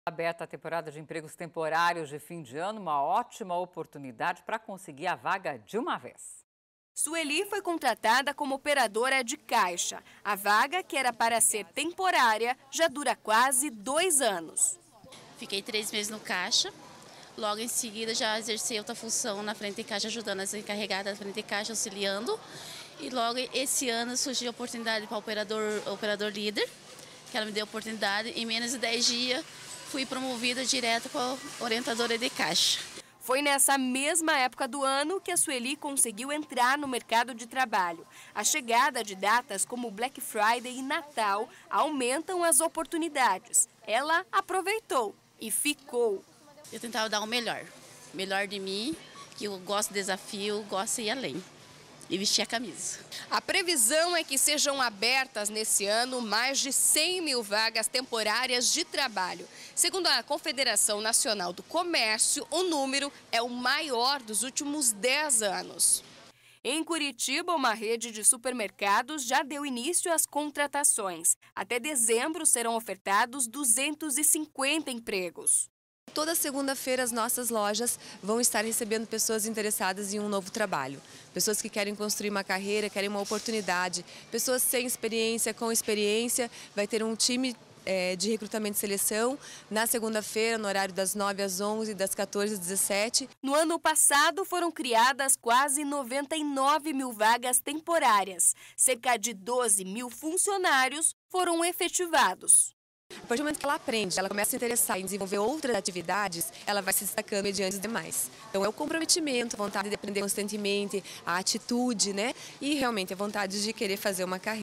Aberta temporada de vagas temporárias no mercado de trabalho
Matéria exibida no Band Cidade 1° edição do dia 20 de outubro.